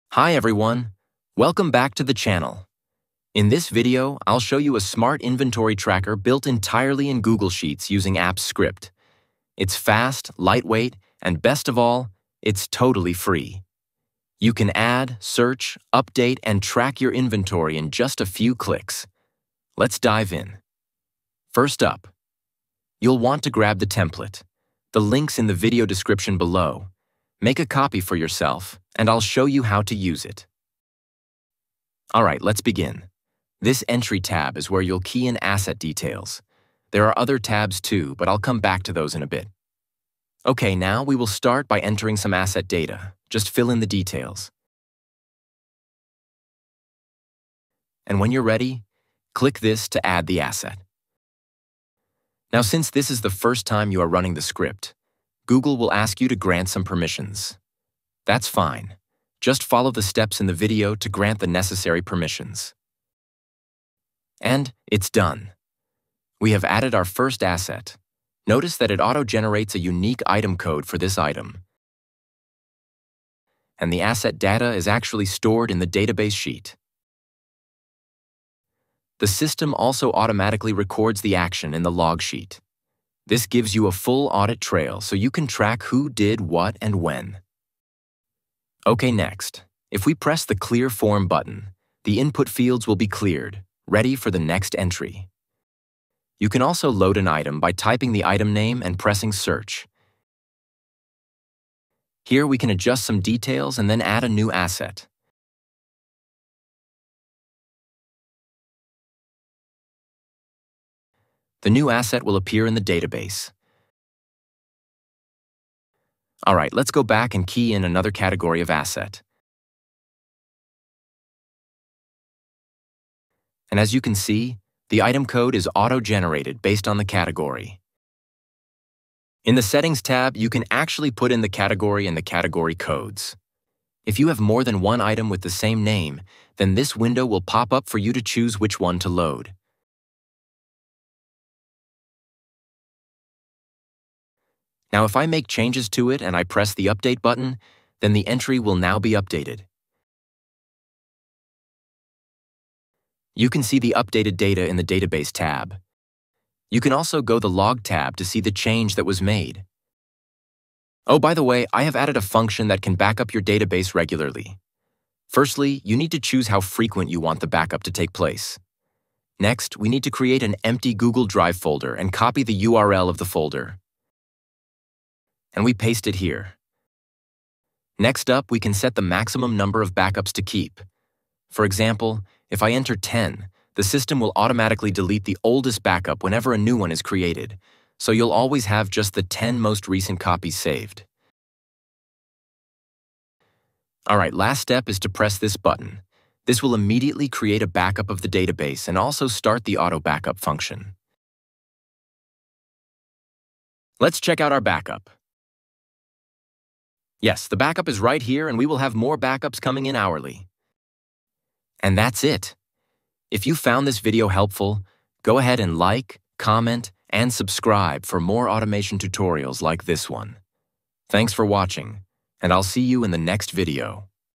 Smart Inventory Tracker in Google Sheets
Manage your inventory like a pro without expensive software or complicated tools!
In this video, I’ll show you how to use a Smart Inventory Tracker in Google Sheets, powered by Google Apps Script. With this system, you can:

-Search items by serial number, name, category, or item code
-Auto-generate unique item codes based on category
-Add and update inventory records in one click
-Maintain a detailed audit log of every change
-Schedule auto-backups

This system is perfect if you:
- Manage a school or department’s assets
- Run a small business with assets
- Or just want a better way to track stuff at home